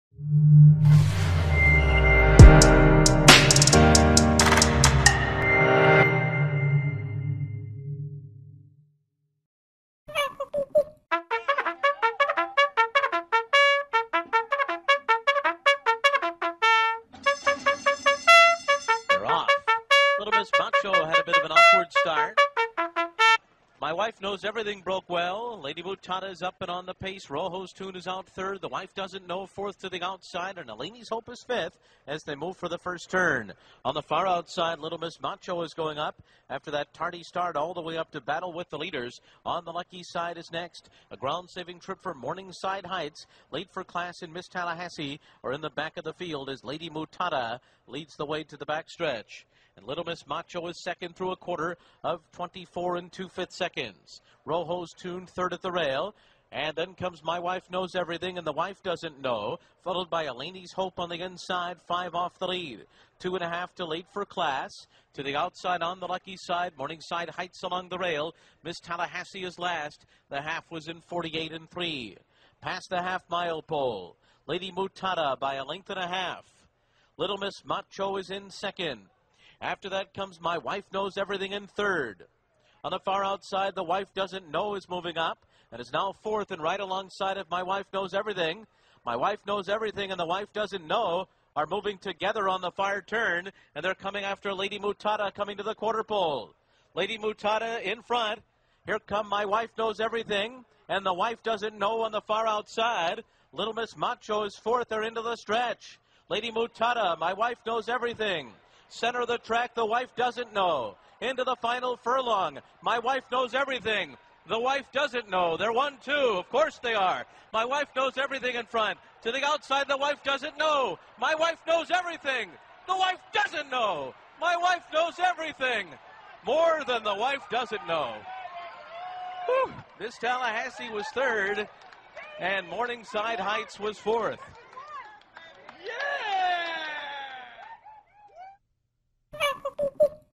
Funny Horse Race Commentary - Funny Monkey Media
The names of these horses are hilarious. The commentator is priceless trying to keep up with these horses names.

Creative Commons Attributes:
"Horse Race With Funny Names" by BEST SPORT VIDEO of YouTube.com